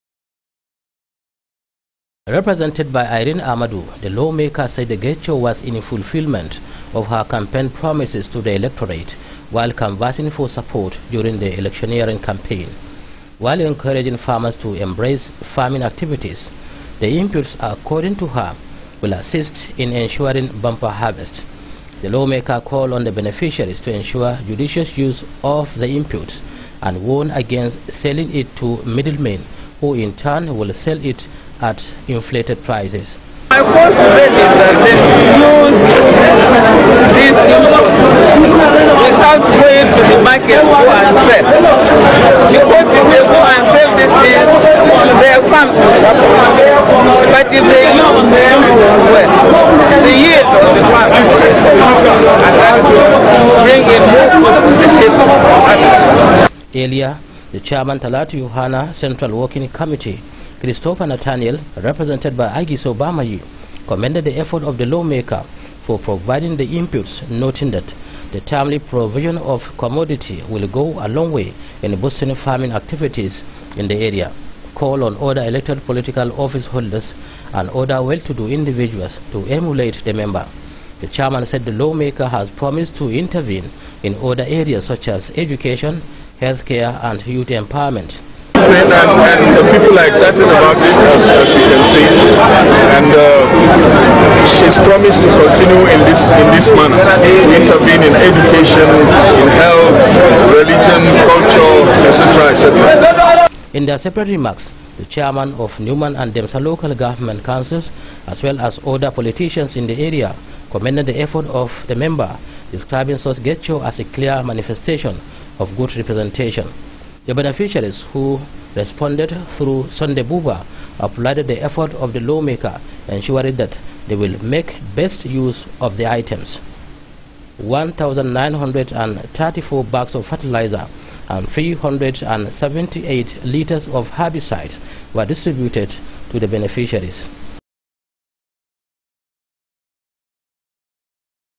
FERTELIZER DISTRIBUTION NUMAN
The people of Demsa, Numan and Lamurde Federal constituency have been advised to embrace farming in order to boost food production in the area.